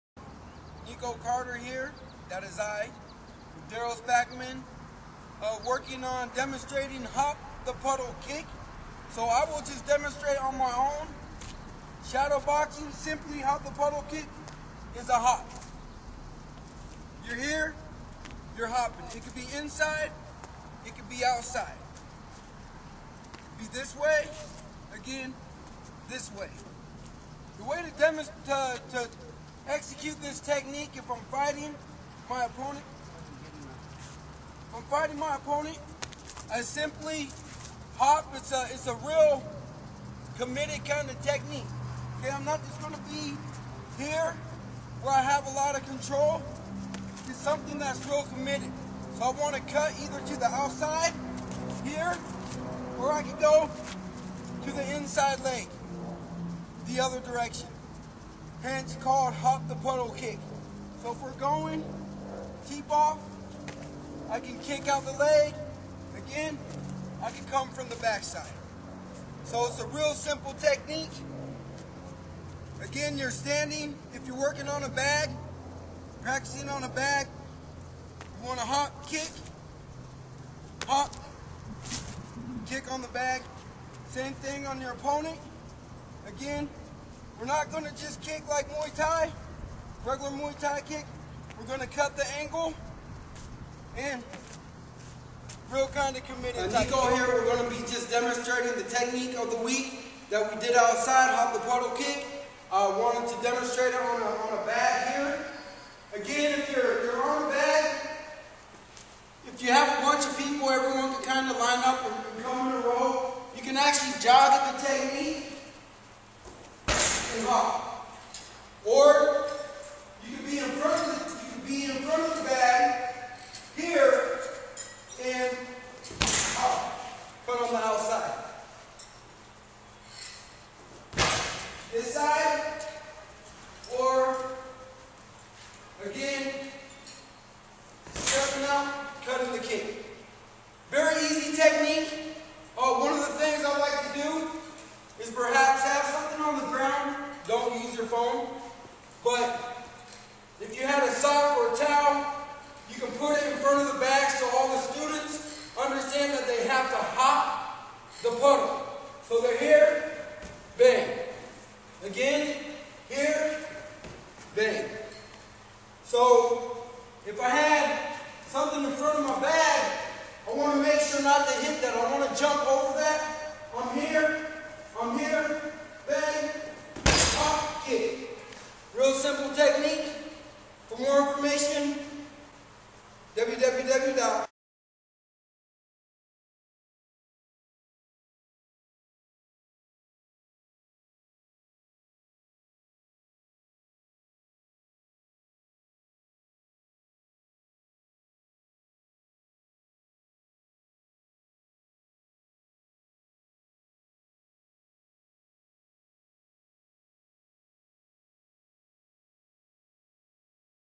RoadworkTraining.com | "Hop the Puddle" Kick
RoadworkTraining.com / Muay Thai Technique of The Week: "Hop the Puddle" kick here at Roadwork Training .com ! This is a simple technique that should be used sparingly and should be set up with punches or other techniques unless you have superb timing! This Thai-kick is quite powerful though fairly risky since it is a full committed kick and leaves you momentarily vulnerable.